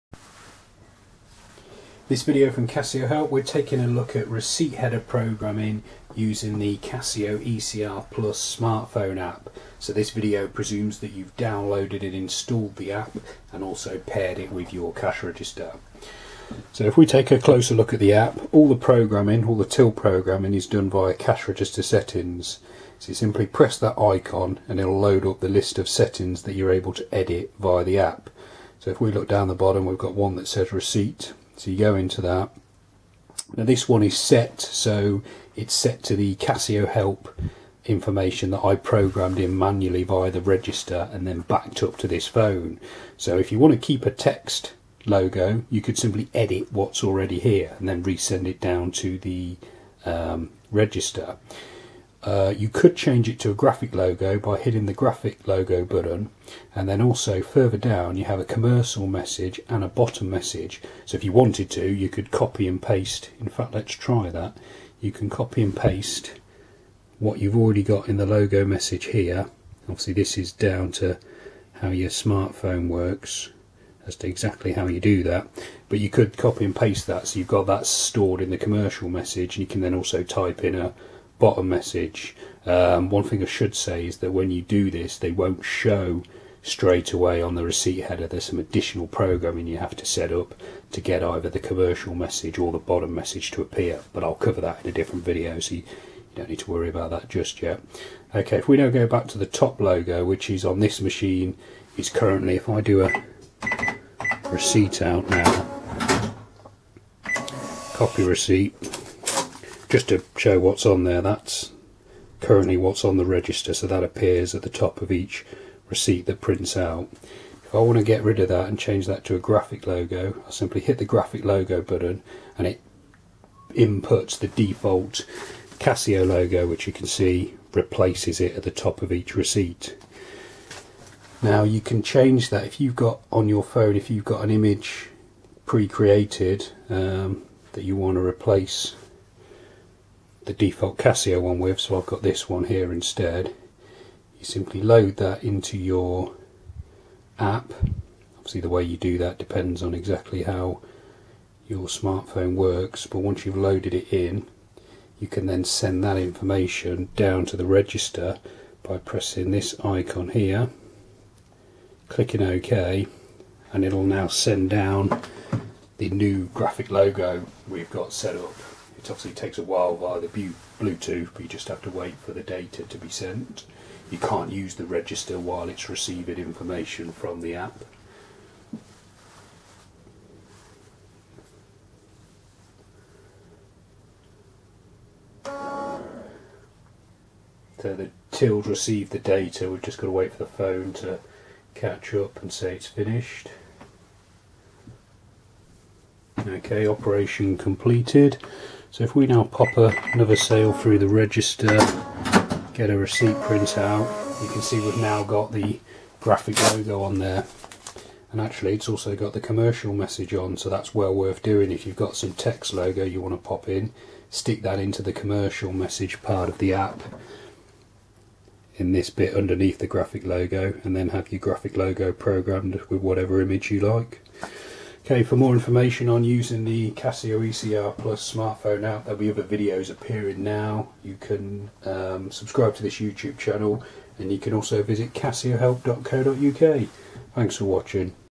How To Program Graphic Logo Receipt Header On Casio Cash Register With Casio ECR+ Smartphone App
How to program graphic logo receipt header on Casio cash registers,